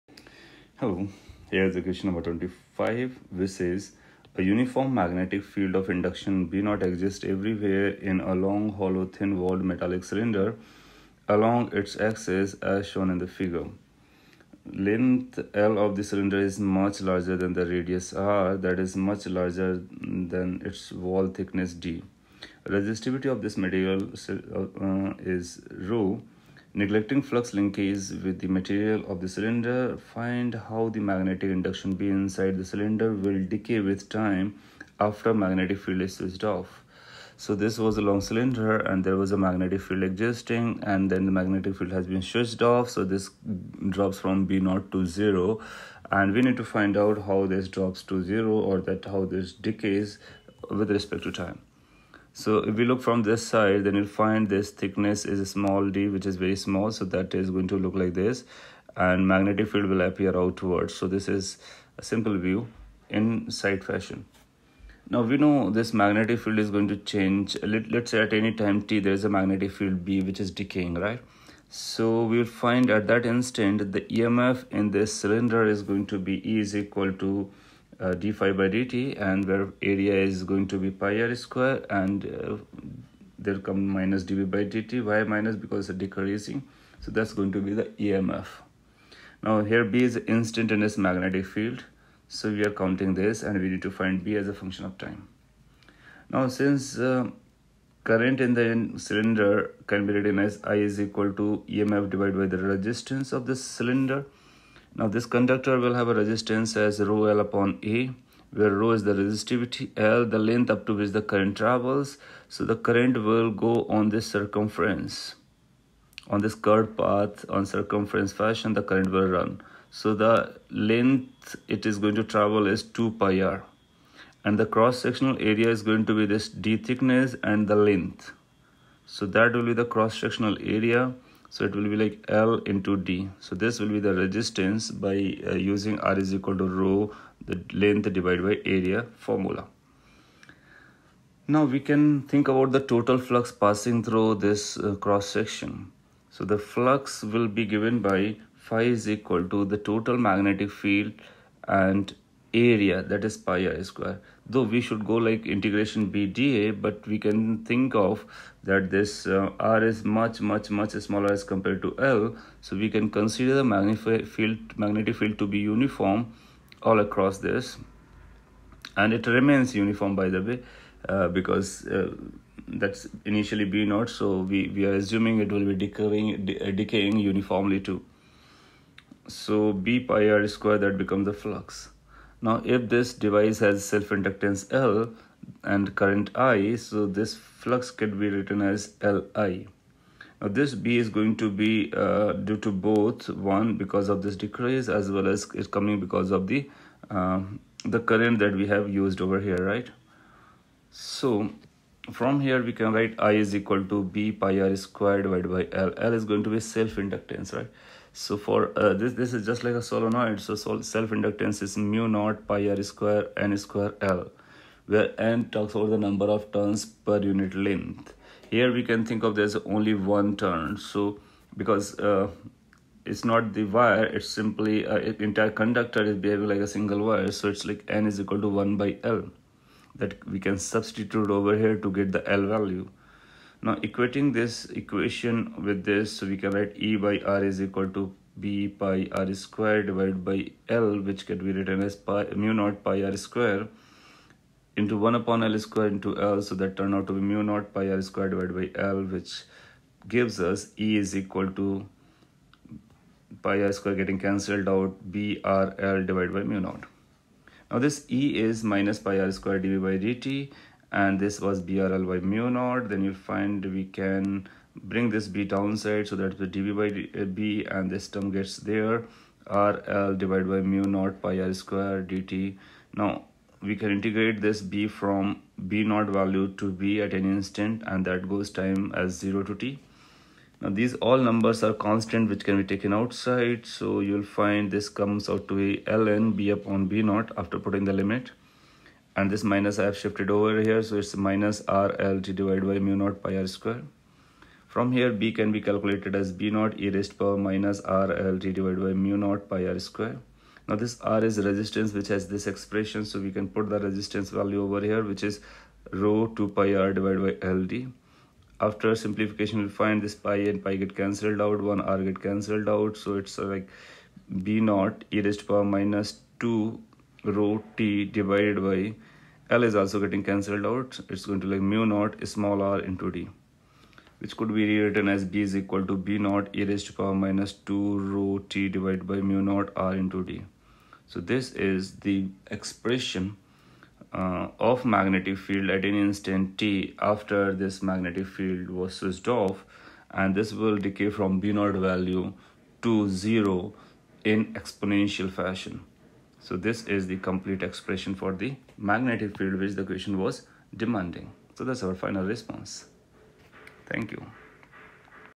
PF EMI BYU 25
A uniform magnetic field of induction Bo exists everywhere in a long hollow thin walled metallic cylinder along its axis as shown in the figure. Length l of the cylinder is much larger than its radius r that is much larger than its wall thickness d. Resistivity of the material of the cylinder is rho. Neglecting flux linkage with the material of the cylinder, find how the magnetic induction B inside the cylinder will decay with time t after the magnetic field is switched off.